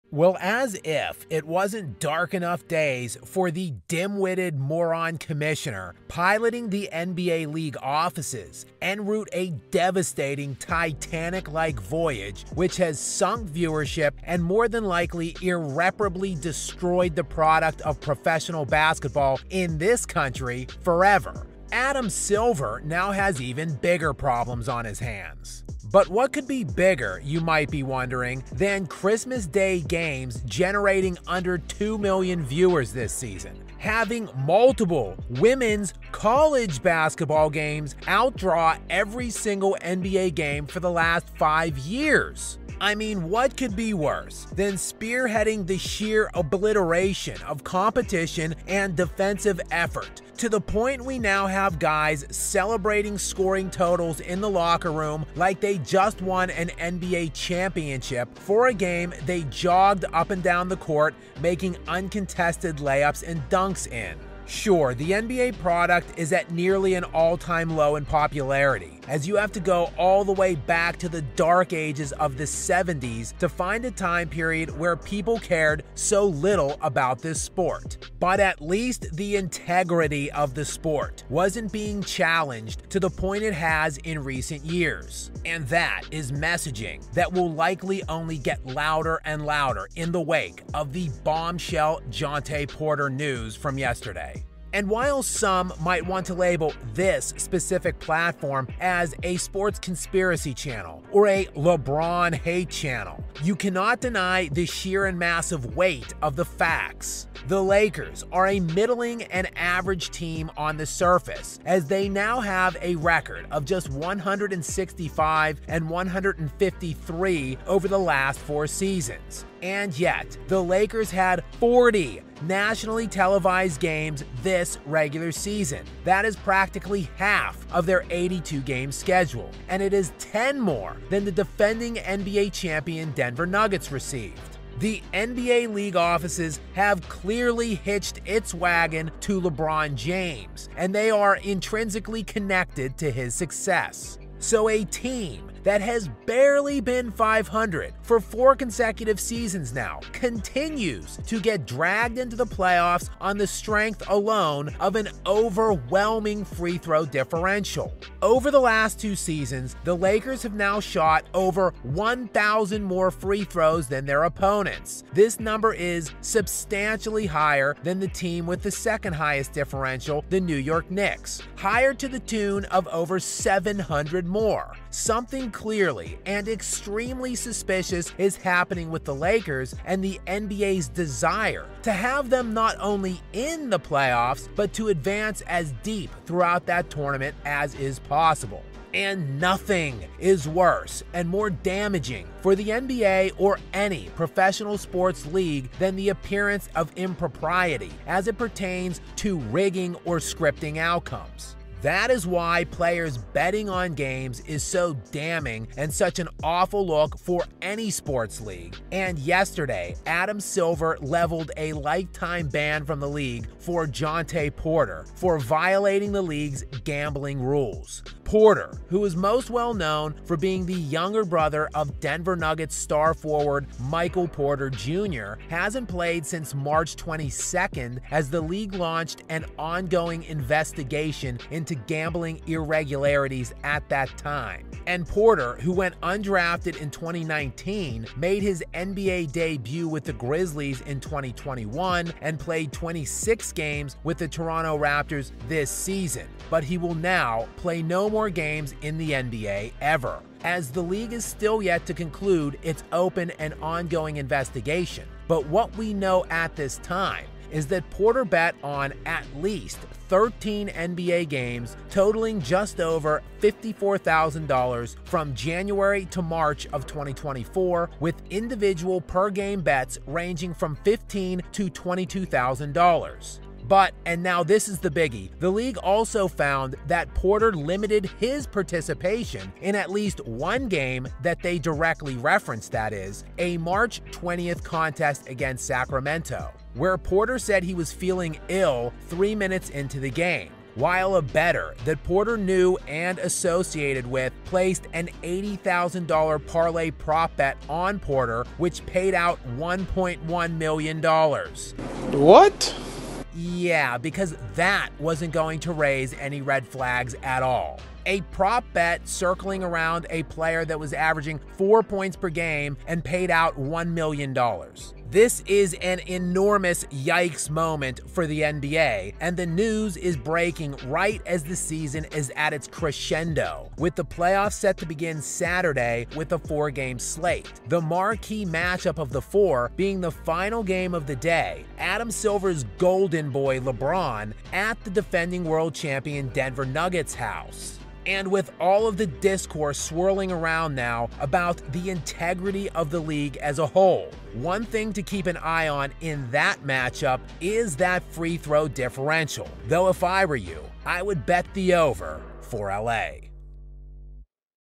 The NBA has a MASSIVE Problem
In the wake of Jontay Porter's lifetime ban from the NBA, questions swirling around the integrity of the game will only get louder.